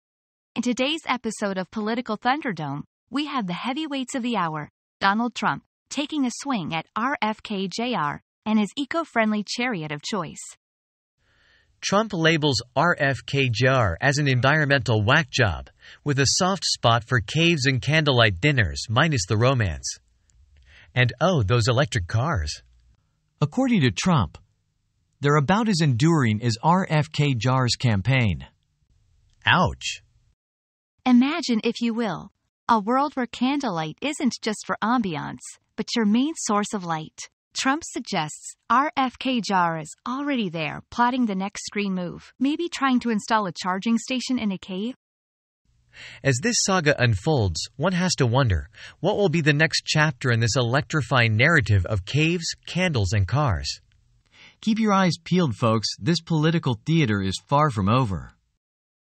Trump vs RFK Jr.: A Battle of Wits or Witless?
Dive into the comedic clash between Donald Trump and RFK Jr. over electric cars, environmental policies, and presidential campaigns. Who needs soap operas when you have politics?